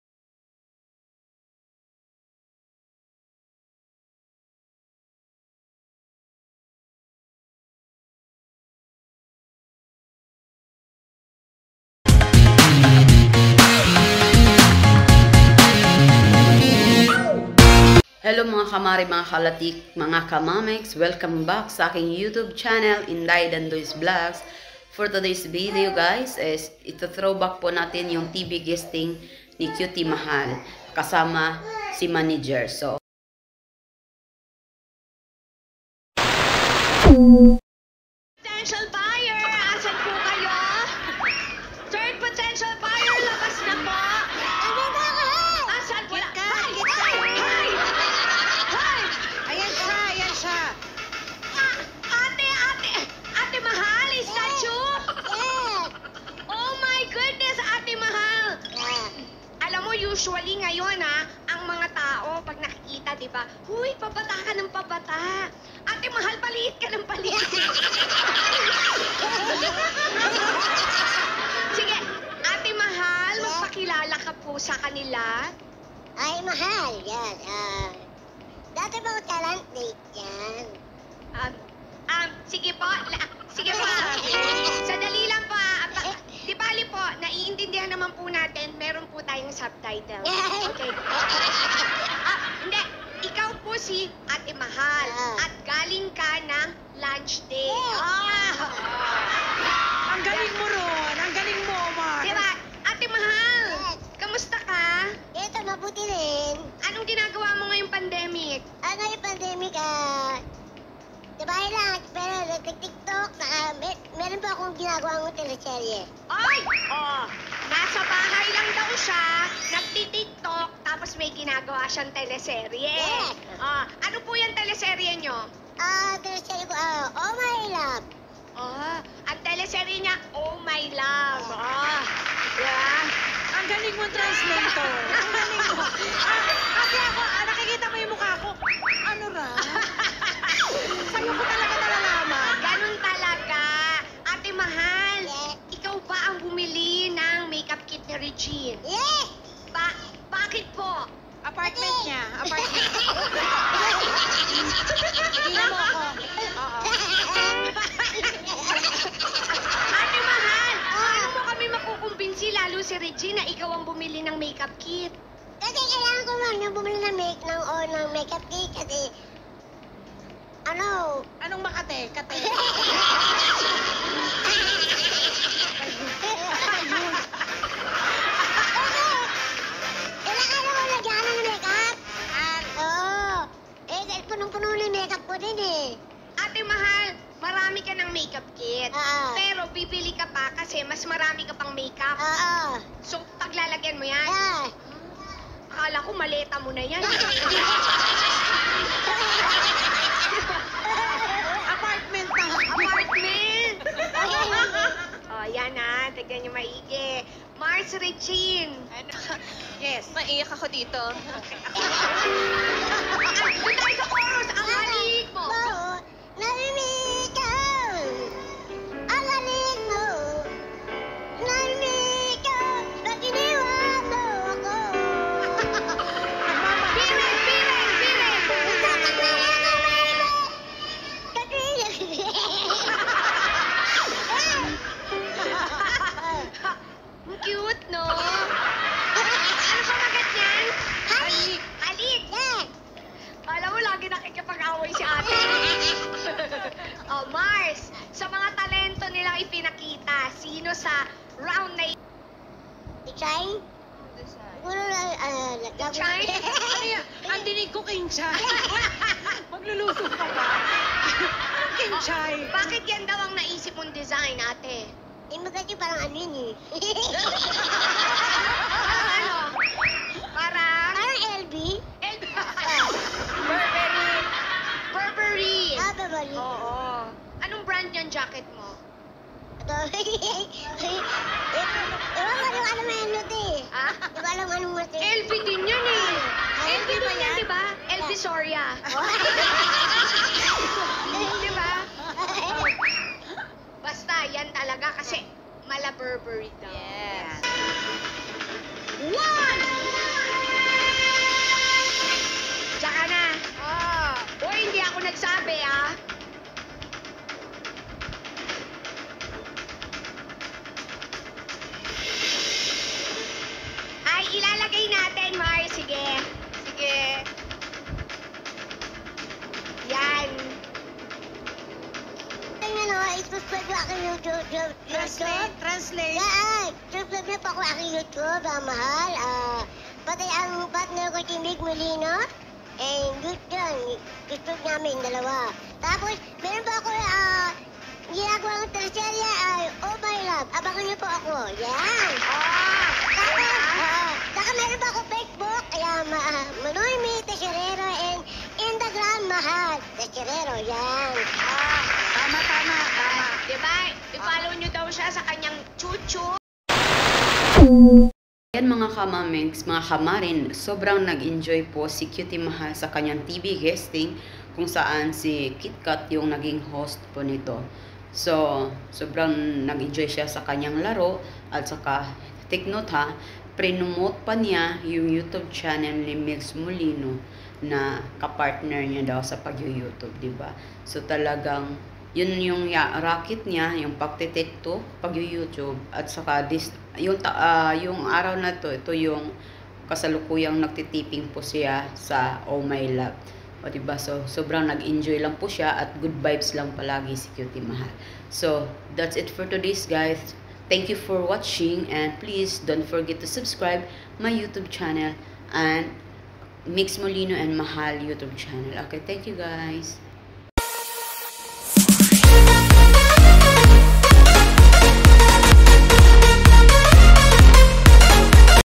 TV GUESTING NI CUTIE MAHAL ATING BALIKAN!
sobrang nakakamiss na talaga ang goodvibes ni cutie mahal..walang tao ang hindi tatawa pag si cutie mahal na ang nagpapatawa..